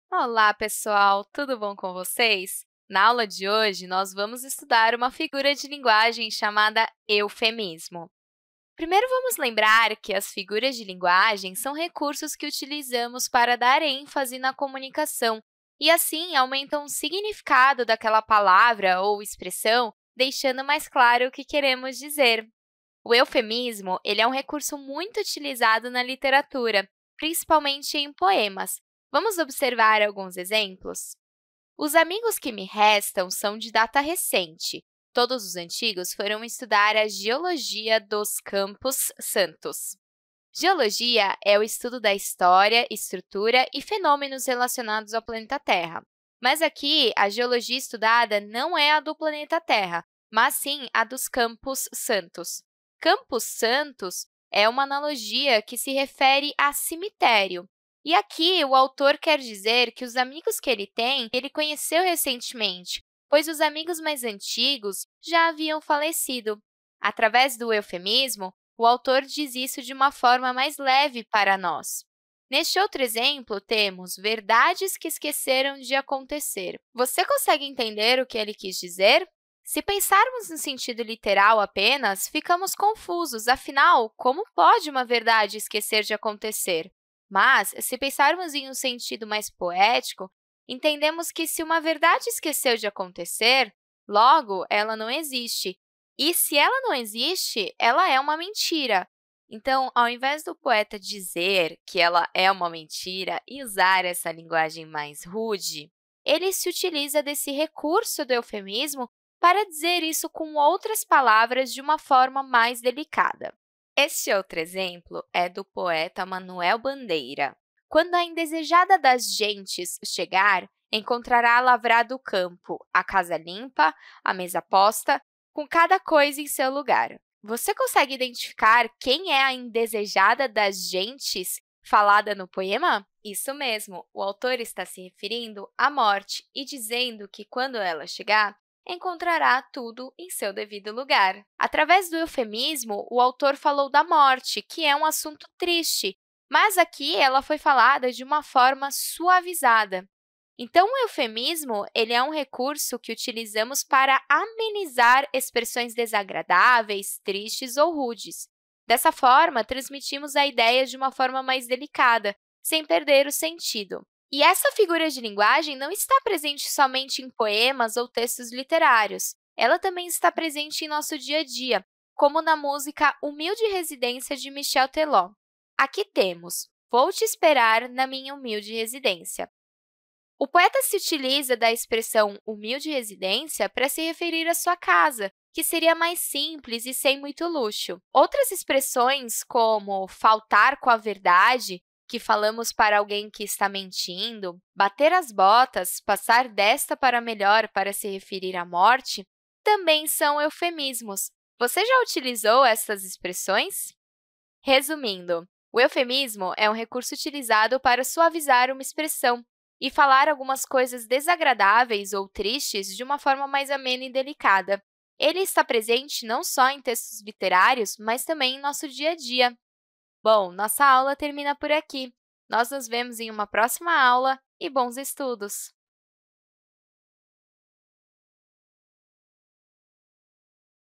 Figuras de linguagem: eufemismo
Nesta videoaula, apresentamos informações sobre o eufemismo, que é considerado uma figura de linguagem. Além do conceito e de exemplos, também é feita uma análise para contribuir para o desenvolvimento da habilidade de interpretar textos e efeitos de sentido.